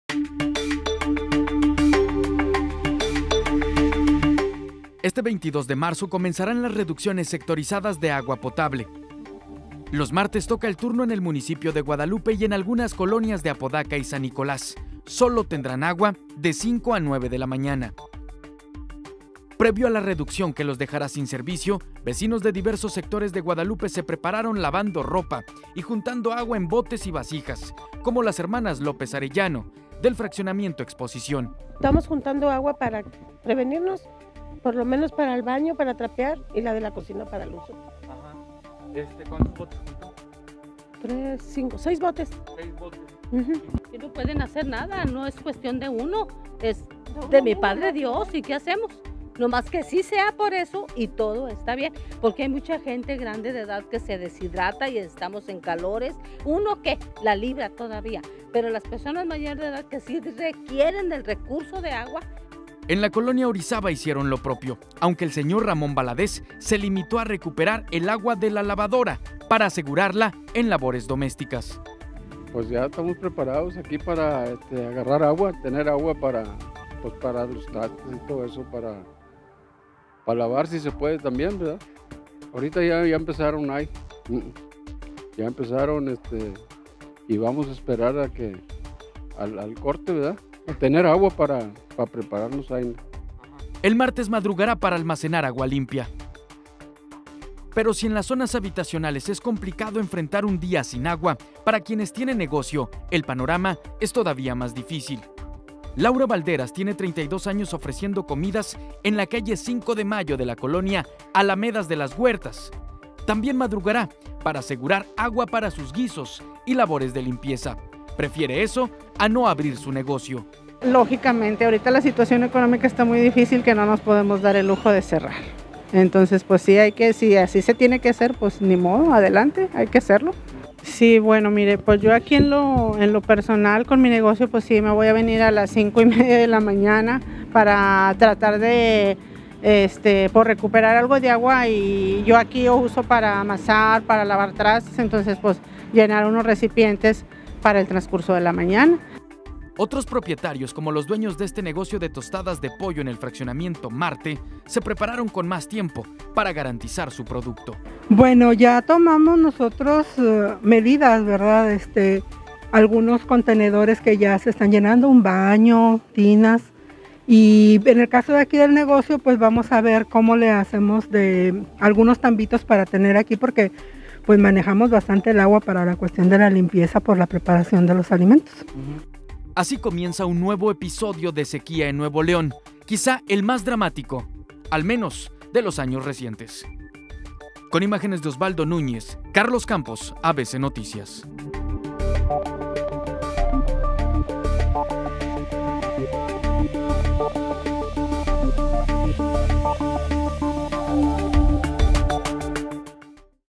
Comienzan reducciones sectorizadas de agua potable
Este 22 de marzo los vecinos del municipio de Guadalupe y algunas colonias de Apodaca y San Nicolás se prepararon para lavar ropa y almacenar agua en botes y vasijas.